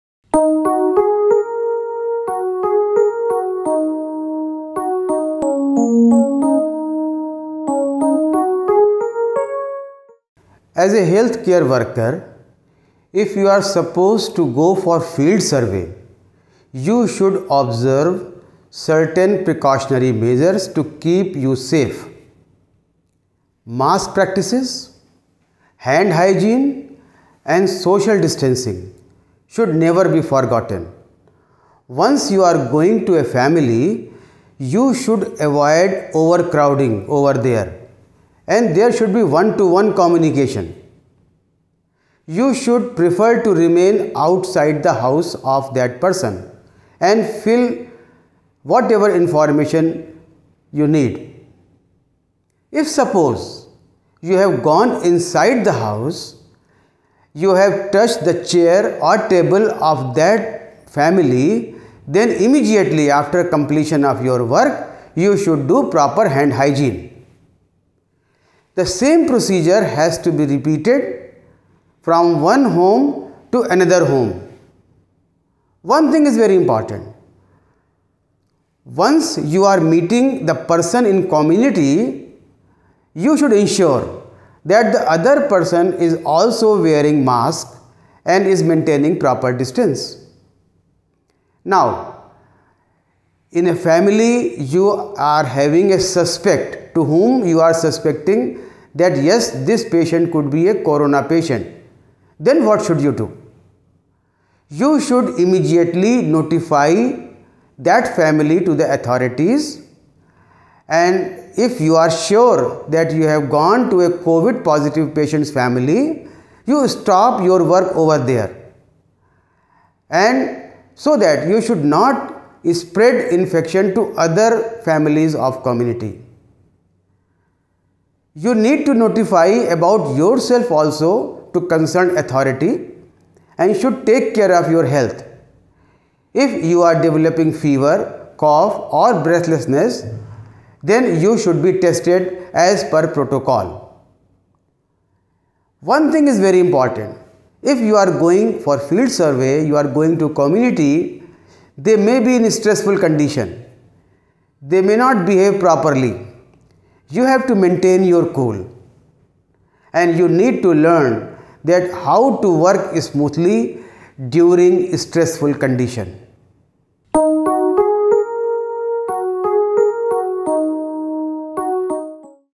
Precautions During Field survey # English # KGMU
This video narration is especially important for HCWs who need to do field survey in COVID affected areas. If we follow these basic principles then we will stay protected while performing our COVID duties.